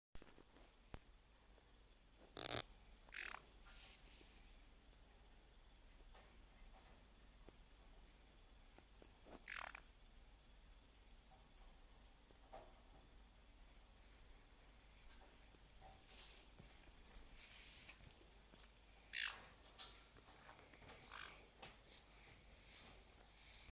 【シロハラインコ ふうすけ】たまにはモフモフにくるまって
ふうすけさん、おっとーのとことに歩いていったらモフモフなひざかけに包まれちゃいました。
意外と気持ちよさそうにしててビックリ。
モフモフもさせてくれましたが、カメラに気づいたらすぐおきちゃったのが残念。

またチャレンジしてみようと思います(´ー`)

毎日Blog 書いてます！